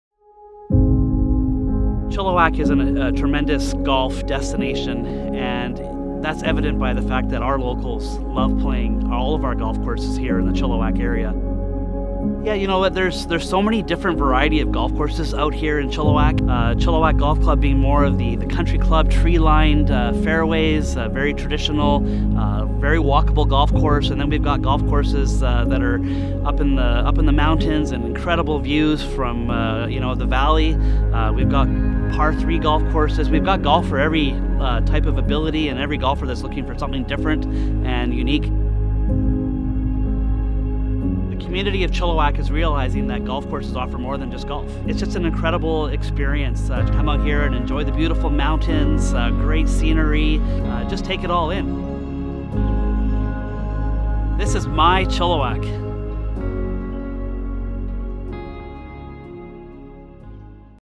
My Chilliwack with Brian Ewart
We are Chilliwack.

My name is Brian Ewart from the Chilliwack Golf Club and this is my Chilliwack.